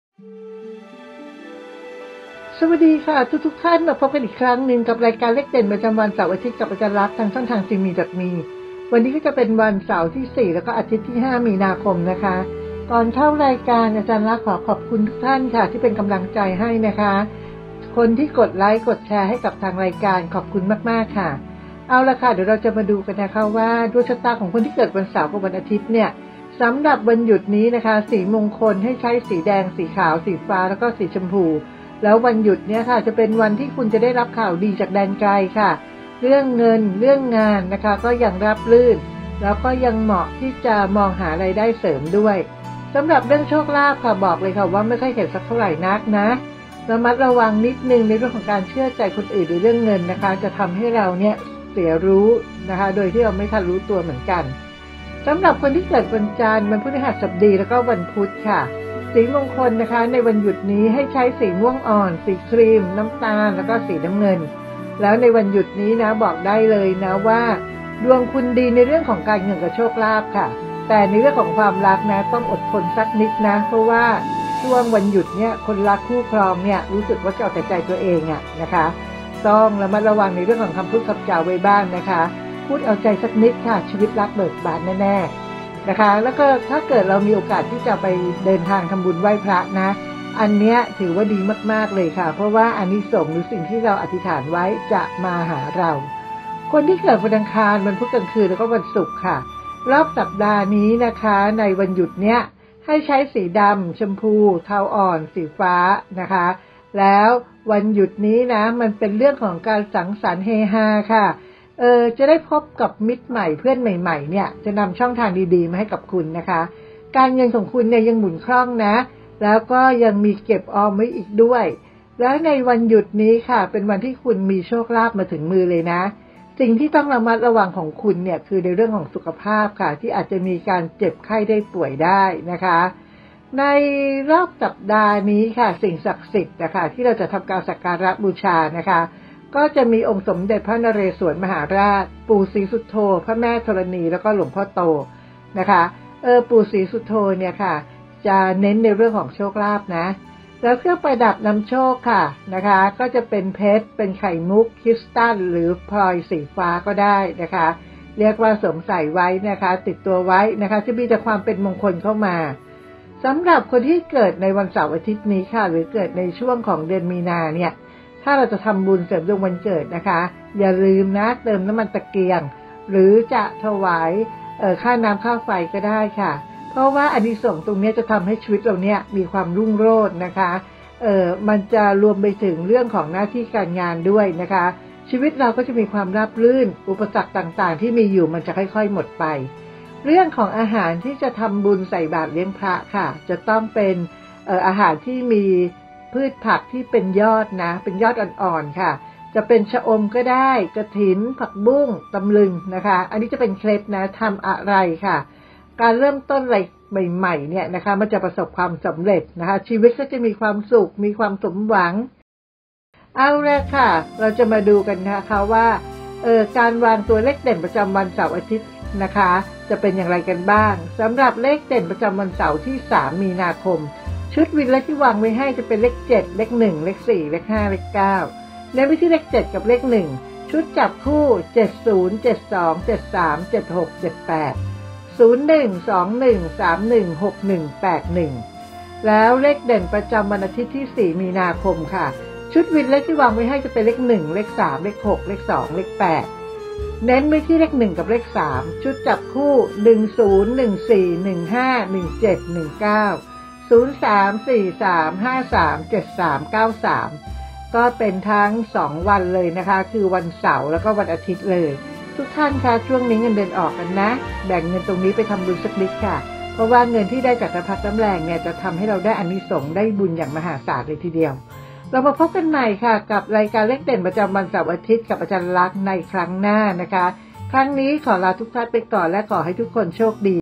เลขเด่นประจำวันเสาร์ - อาทิตย์ที่ 4-5 มี.ค. 66 กับ อ.รักษ์ เลขเด็ด
รายการเลขเด่นประจำวันกับ อ.รักษ์ เลขเด็ด เป็นรายการพยากรณ์ดวงชะตารายวันตามดวงวันเกิด และเลขมงคลเพื่อนำไปใช้ในชีวิตประจำวัน โดยอ.รักษ์ ได้วิเคราะห์และแบ่งตัวเลขให้ตามช่วงเวลา พร้อมทั้งมีเคล็ดลับเสริมดวงโชคลาภในแต่ละวันมาฝากแฟนๆรายการ สำหรับใครที่ชื่นชอบรายการอย่าลืมกดไลด์ กดแชร์ กดติดตามกันด้วยนะครับ

รายการนี้เป็นเพียงแนวทางตัวเลขเด่นในแต่ละวันเท่านั้น ไม่ได้สนับสนุนการเล่นพนันทุกรูปแบบ โปรดใช้วิจารณญาณในการรับชม

ABOUT US

เข้ากลุ่ม FB หวยเลขเด็ด ของแท้ (เอ็มไทย)

เข้ากลุ่ม Line หวยเอ็มไทย

ตรวจผลหวยฮานอยทุกงวดได้ที่นี้